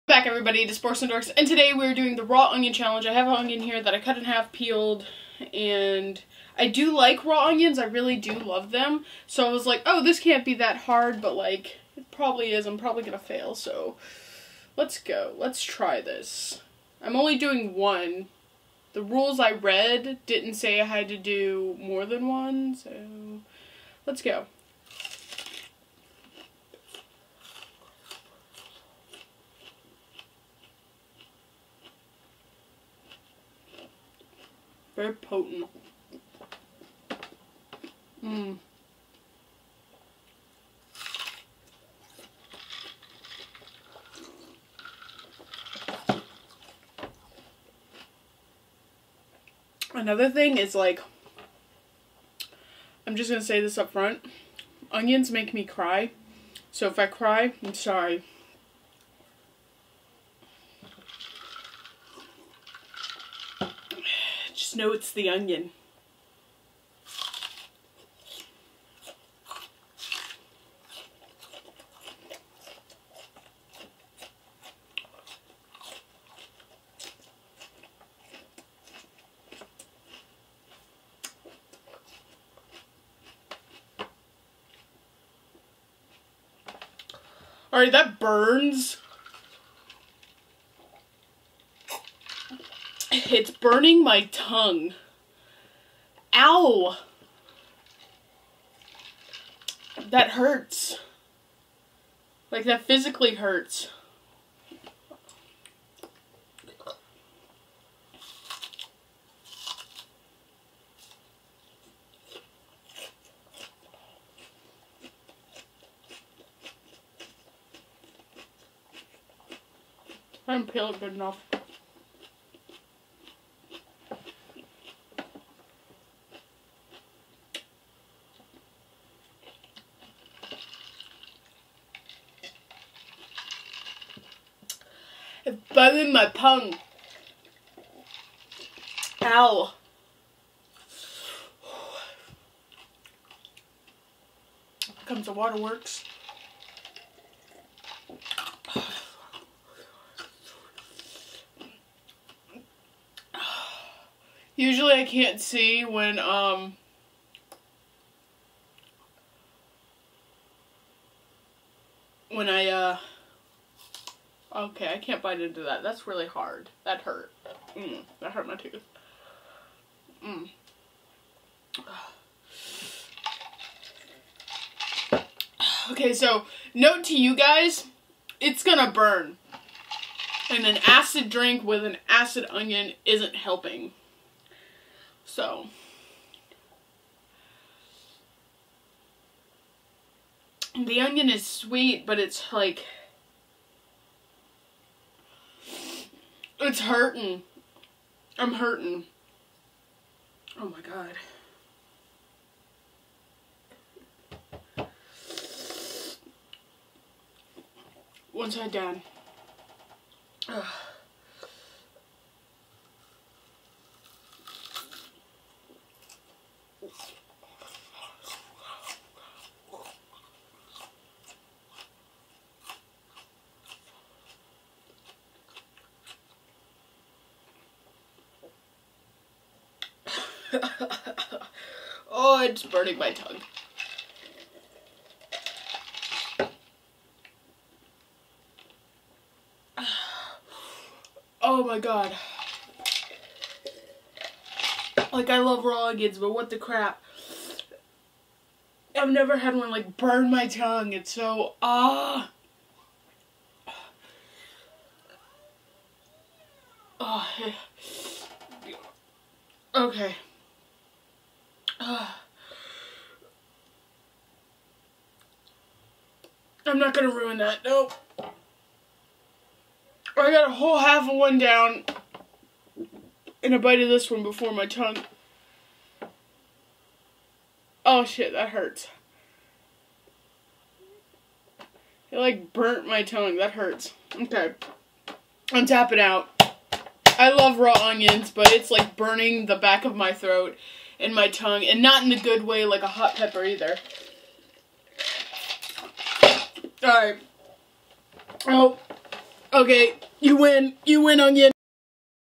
Raw Onion Challenge *FAIL*
Onion challenge rules: Eat one whole Raw Onion as quickly as possible.